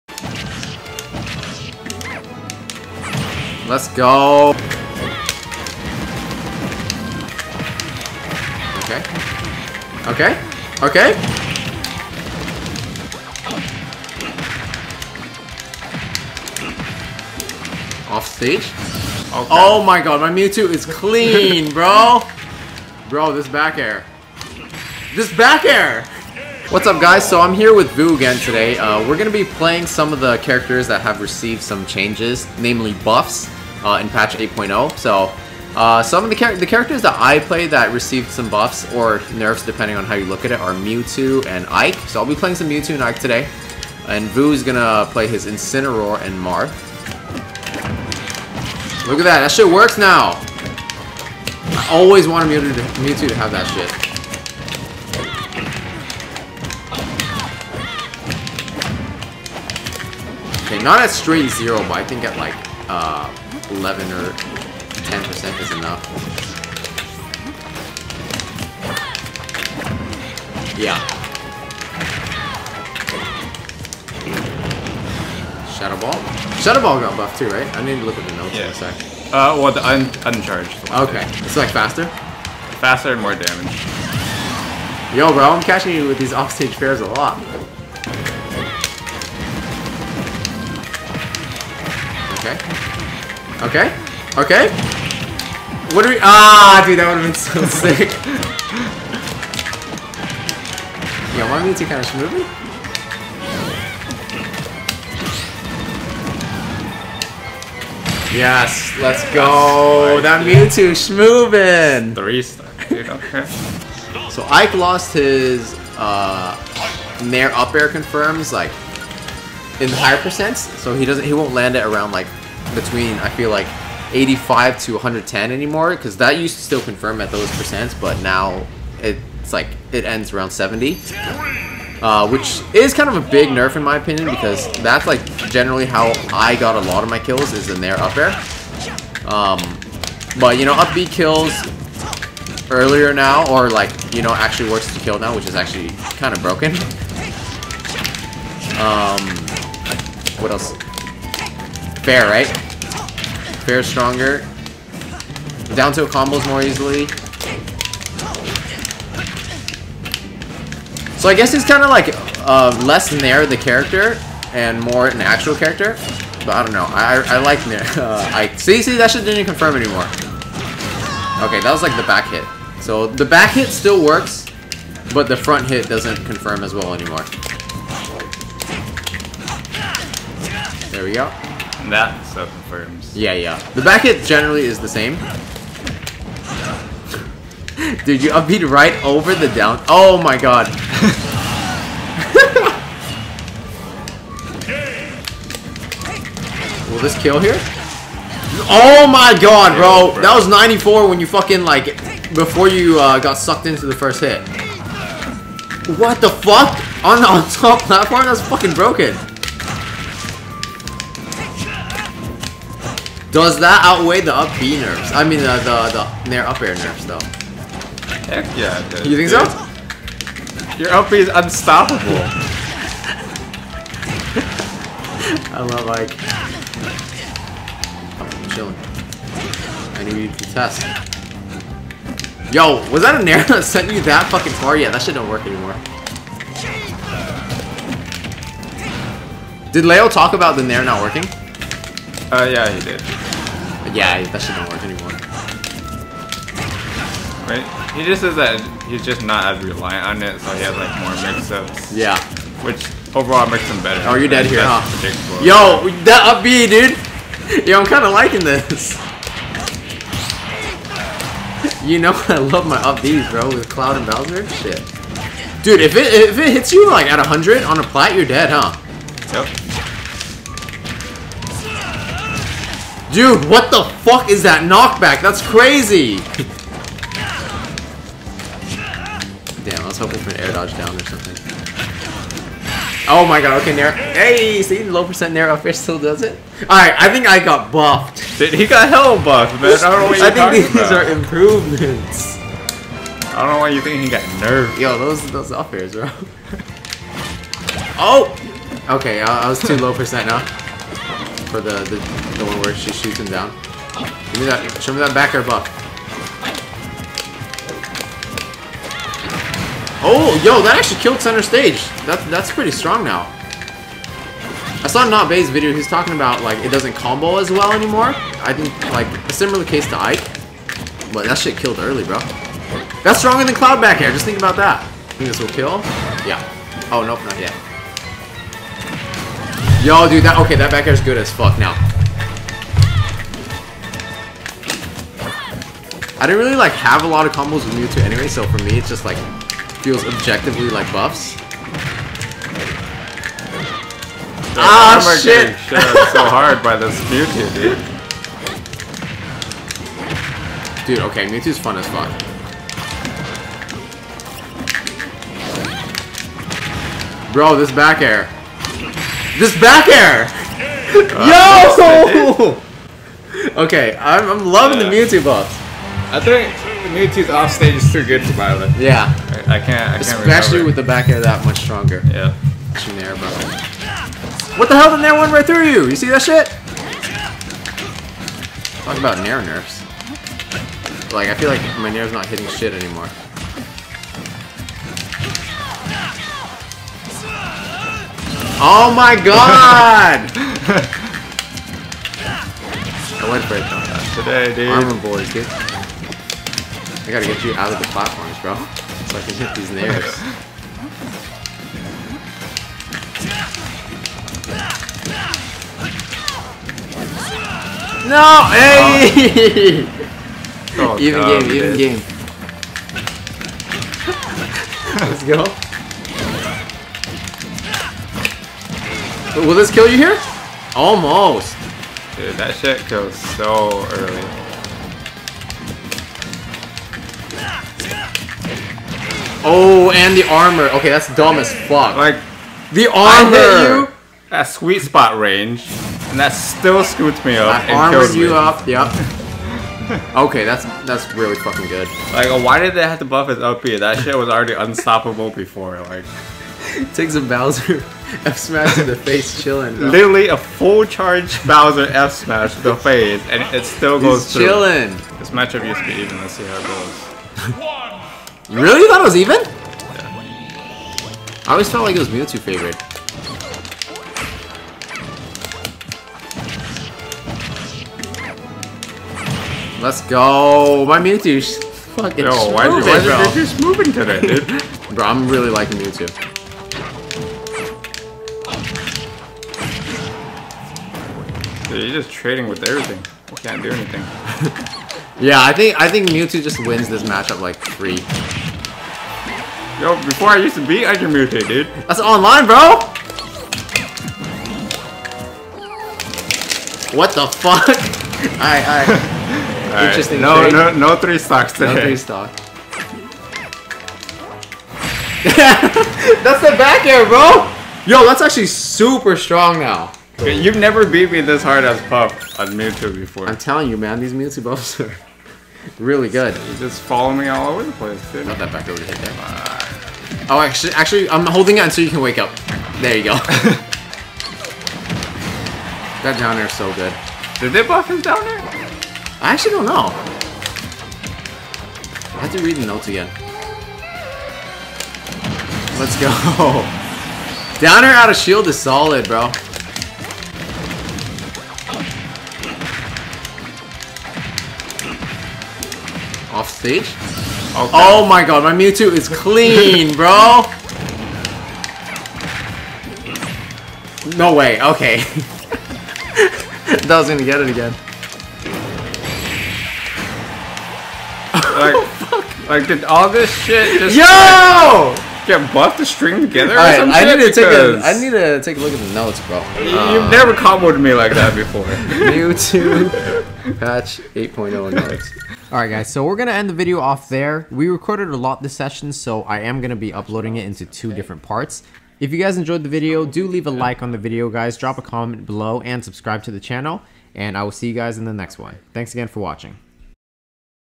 MEWTWO & IKE Patch 8.0.0 BUFFS & CHANGES
Playing around with Mewtwo & Ike after the patch 8.0.0 changes. Buffs in some cases, nerfs in others. Mewtwo is TONS of fun rn!